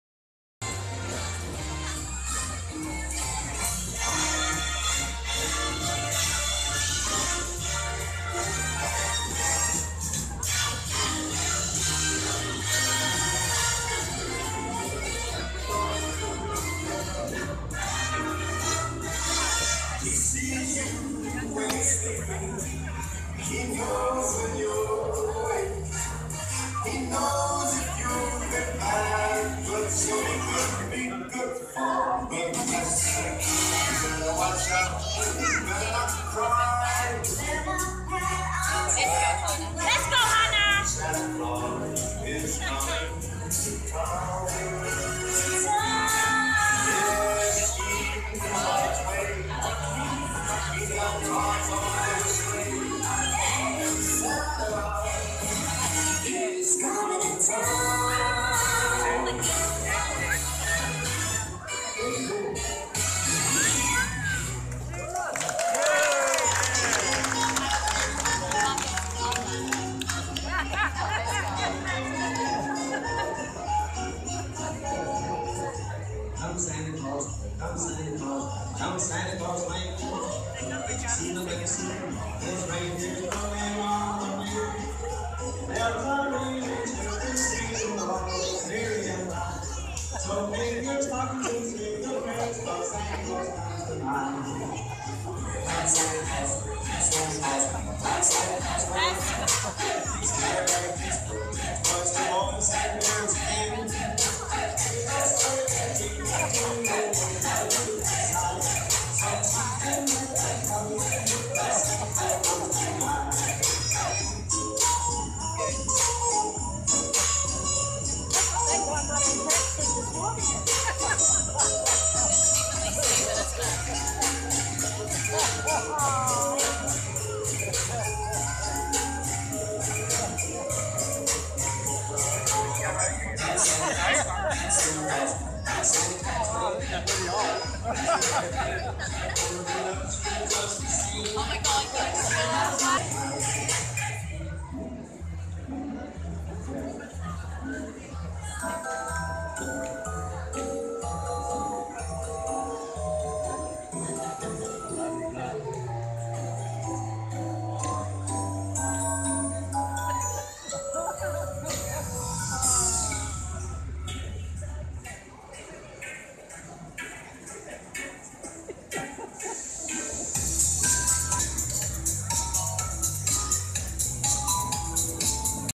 Santa Dance and helpers Christmas special 2018
Xmas Santa Cruz and their helpers girls dance